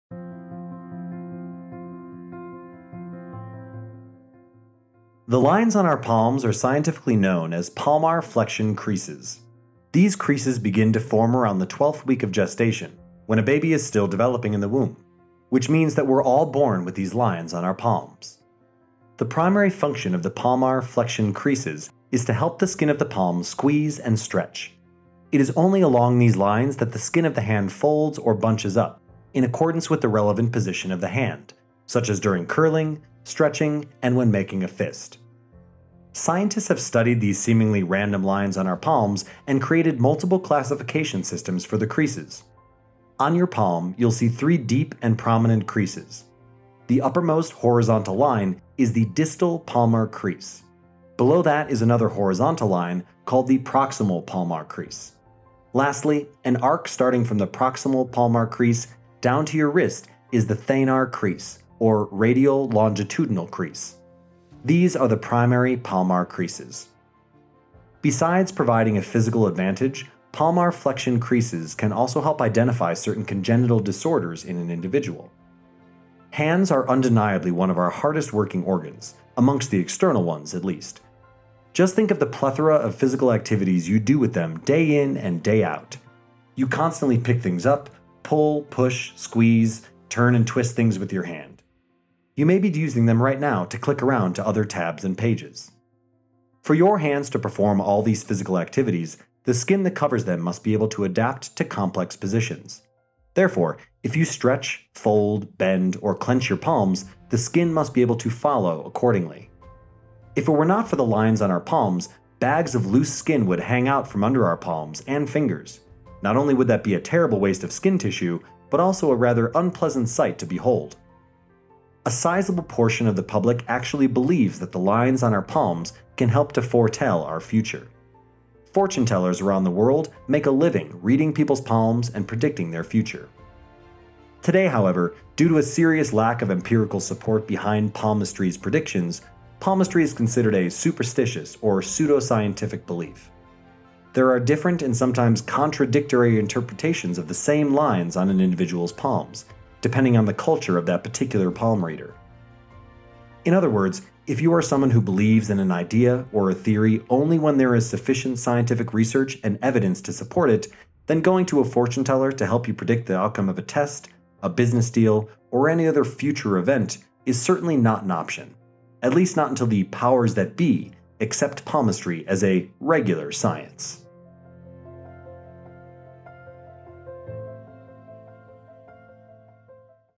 Why Do We Have Lines On Our Palms?
The lines on our palms are known as palmar flexion creases, and they’re there to help us fold, stretch, squeeze, scrunch and do other such things without excessively stretching or squeezing the skin on the hands.

The lines on our palms are scientifically known as palmar flexion creases. These creases begin to form around the 12th week of gestation (when the baby is developing in the womb), which means we are all born with these lines on our palms.

For your hands to perform all these physical activities, the skin that covers them must be able to adapt to complex positions. Therefore, if you stretch, fold, bend or fist your palms, the skin must be able to follow suit.

If it were not for the lines on our palms, bags of loose skin would be hanging out from under our palms and fingers. Not only would that be a terrible waste of skin tissue, but also a rather unpleasant sight to behold.